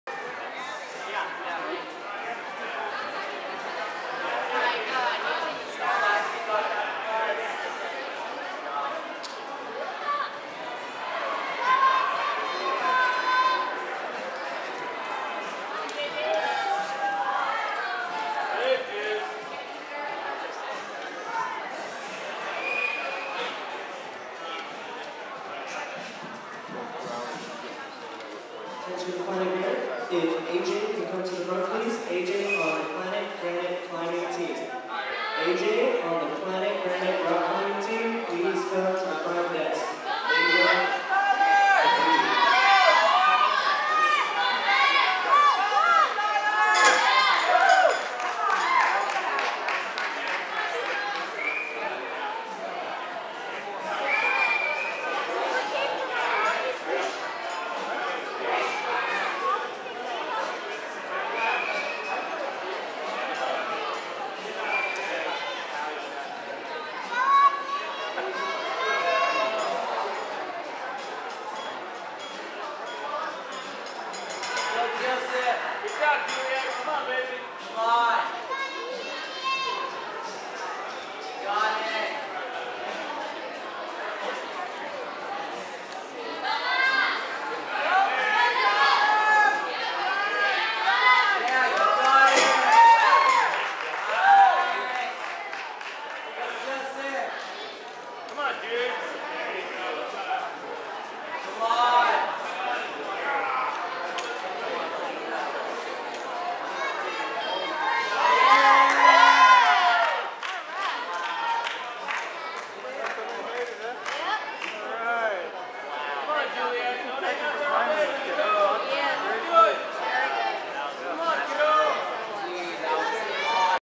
Joe Rocks at 2011 YCL Championships in Belmont CA.
Joseph takes 1st in difficulty, 2nd in speed and 1st overall,